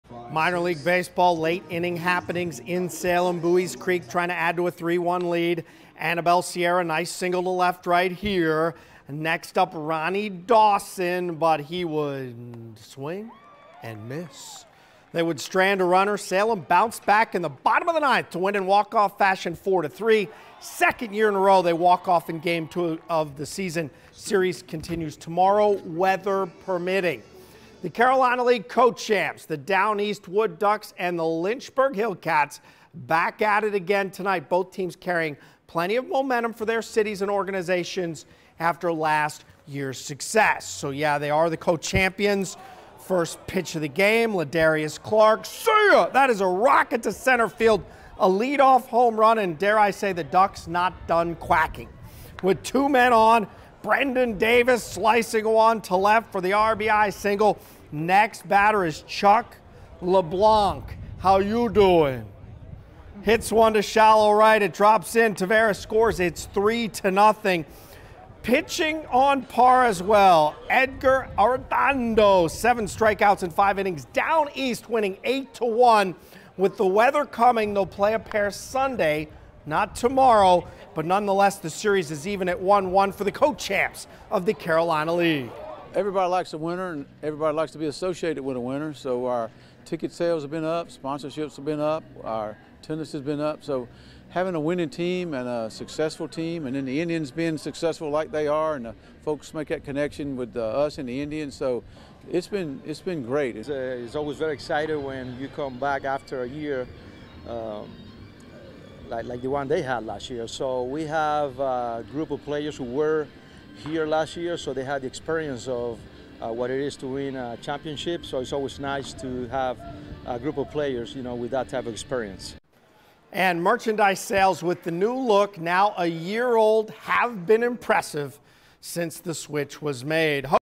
Carolina League Baseball
Salem Red Sox and Lynchburg Hillcats play some Carolina League baseball